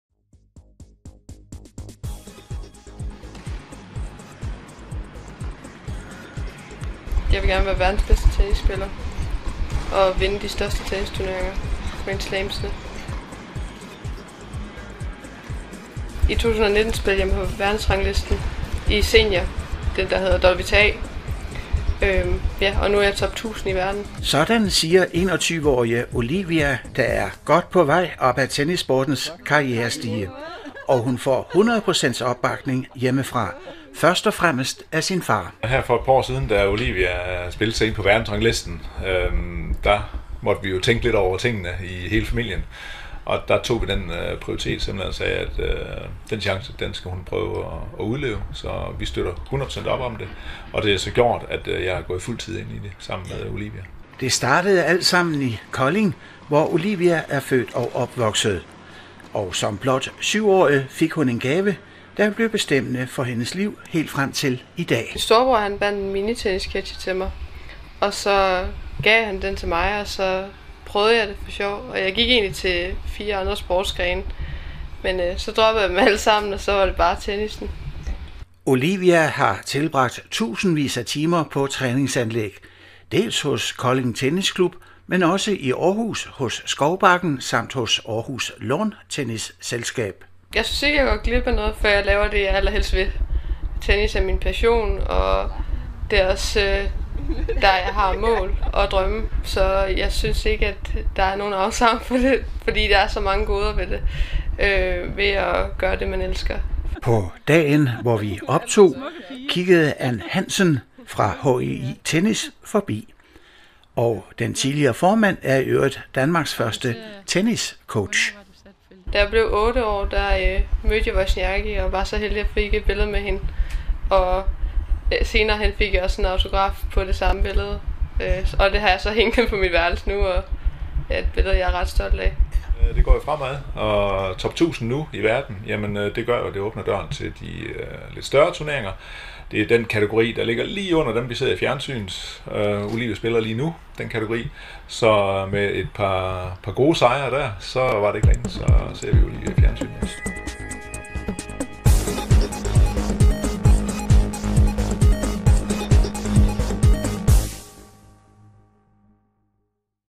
Olivia Gram, blandt de 1000 bedste i verden
Olivia Gram, Tennis. En familie fra Kolding har store tennisambitioner. Faren, Henning Gram, har endog droppet sit arbejde for at kunne støtte sin 21-årige datter, der er et sandt tennistalent. Hun har spillet tennis siden hun var syv, og i 2020 blev hun professionel. Olivia har tilbragt tusindvis af træningstimer på baner i Kolding samt ligeledes hos Skovbakken samt hos ATLS, Aarhus Tennis Lawn-Tennis Selskab. Ligeledes har Olivia rejst i Såvel Asien som USA og har gået på tennis akademi Frankrig